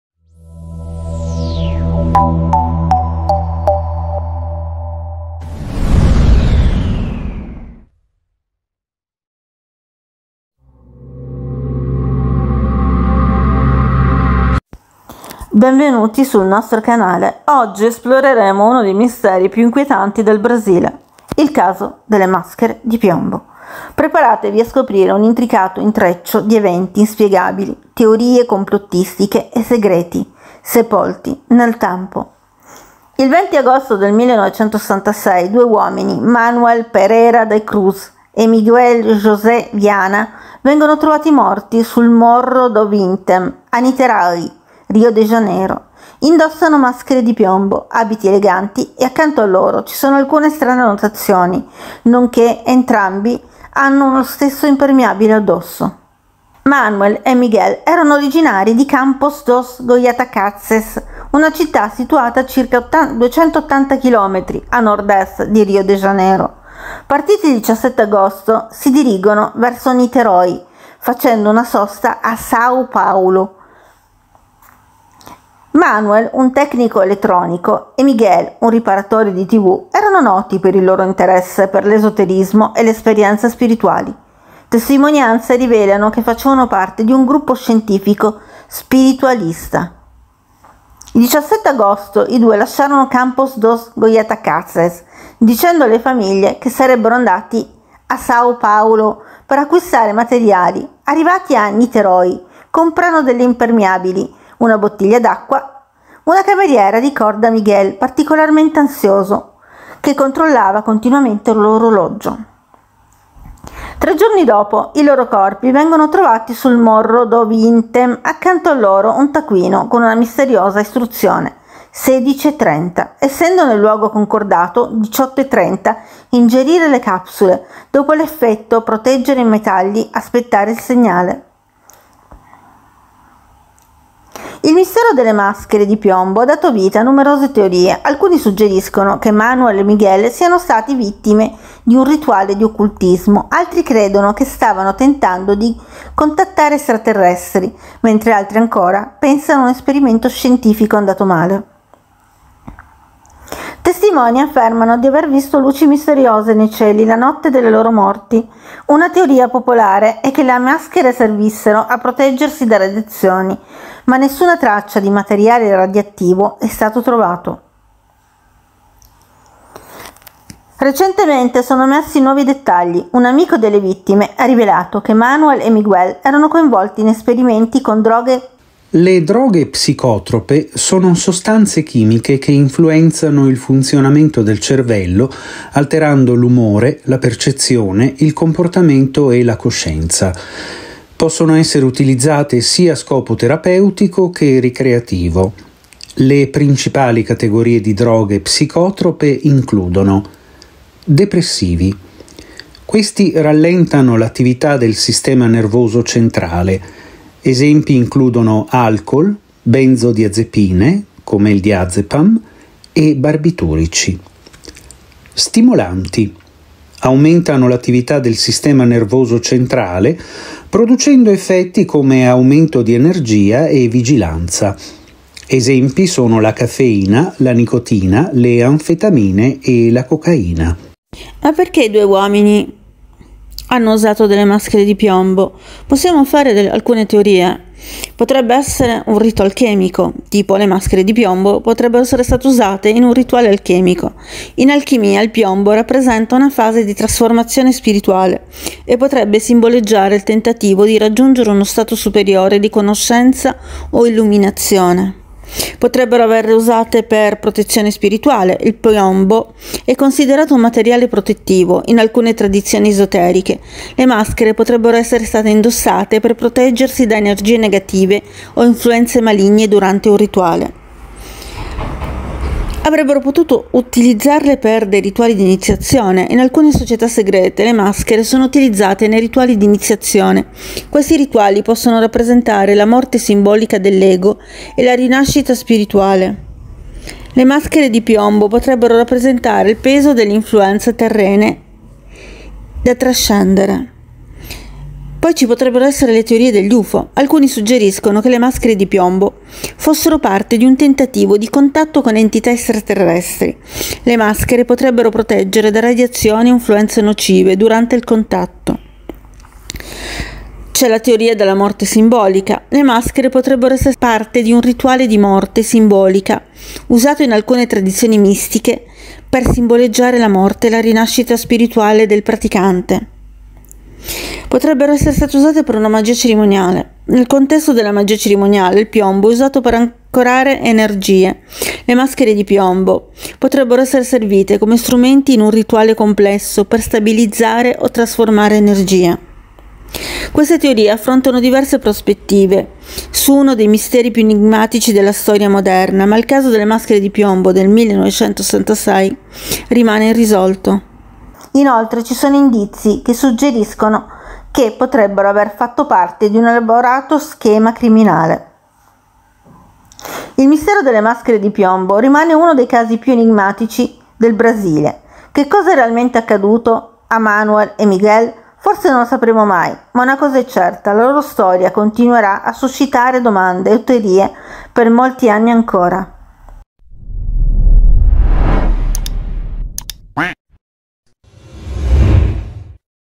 Le maschere di piombo del 1966
Nel 1966, due ingegneri brasiliani furono trovati morti con misteriose maschere di piombo. Questo caso irrisolto ha scatenato molte teorie, da rituali esoterici a contatti extraterrestri. Esploriamo insieme il mistero delle "maschere di piombo" e le possibili spiegazioni dietro questo enigma .